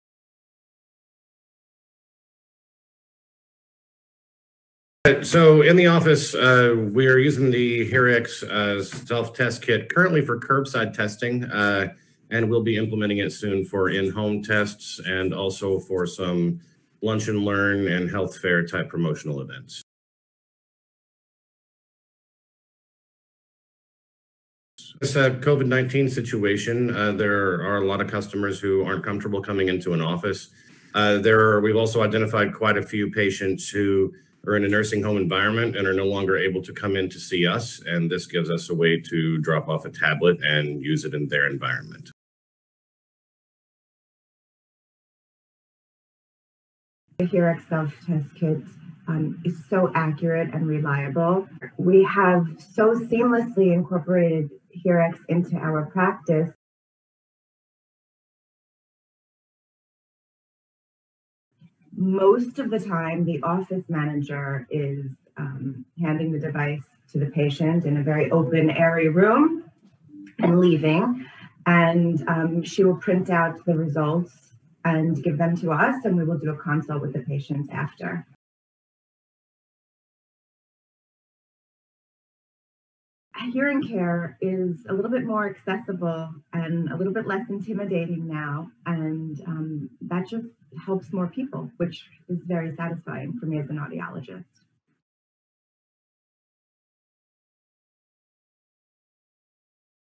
hearX Self Test Kit | Testimonial
The hearX Self Test Kit, a low- and no-touch audiology solution.Hear how hearing care professionals deliver quality hearing care during COVID-19 with the Self Test Kit.